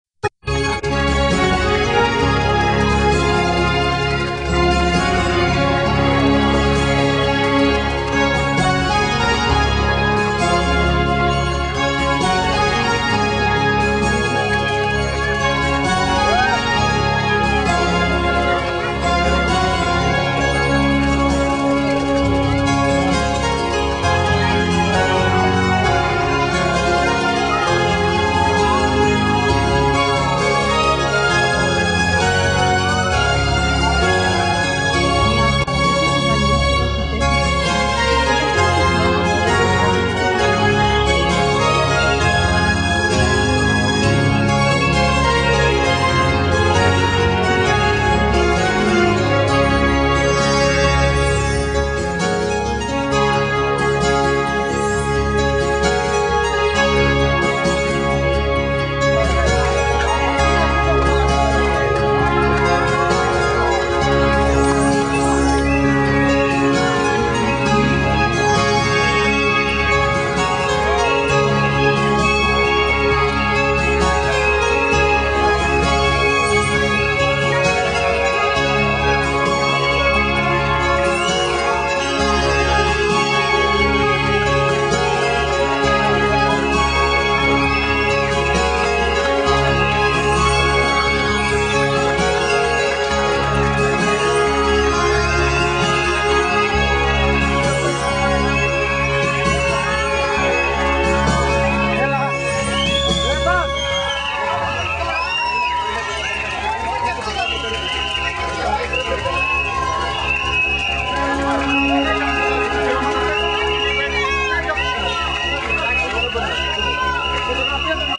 Evan Tsourtsoulas carrying the Olympic Flame
Evan carrying the Olympic Flame in Kastoria 2004